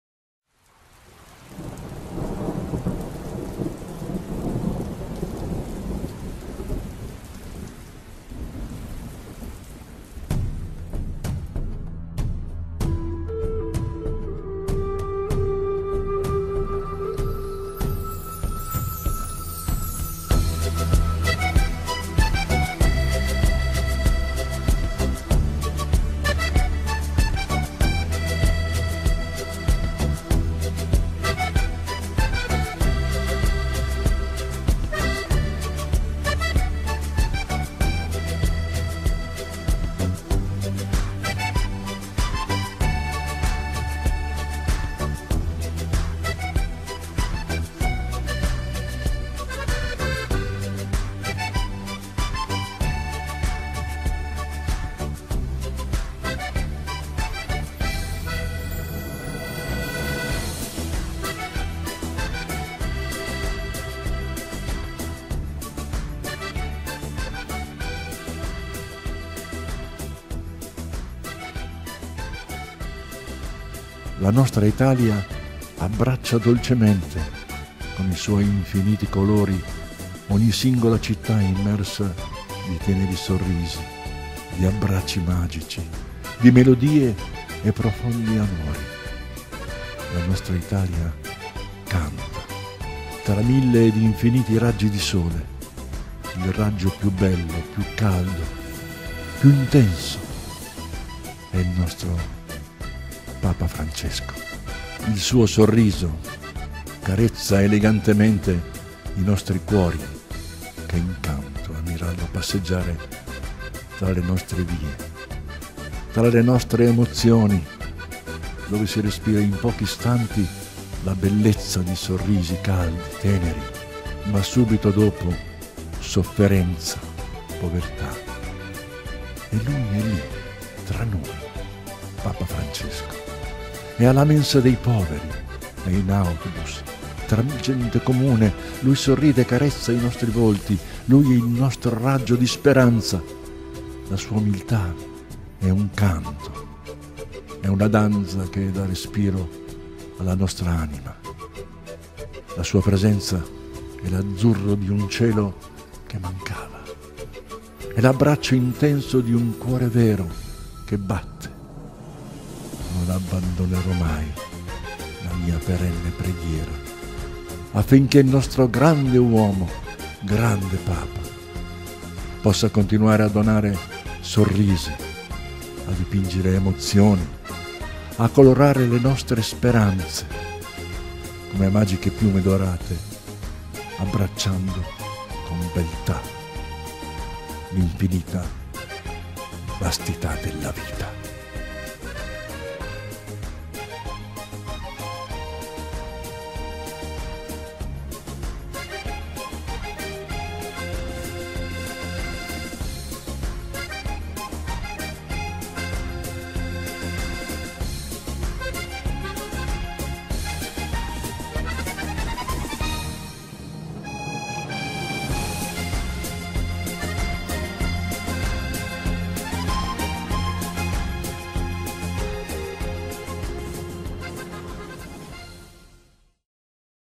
"Un cammino d'amore" di Teresa Mancuso. Voce Nino Castorina. video poesia Paolo Nel Vento.
Poesia di Teresa Mancuso
dedicata a Papa Francesco
inserita nel volume " Da Francesco a Francesco "
casa editrice:  SANTA MARGHERITA LIGURE
edizioni Tigulliana.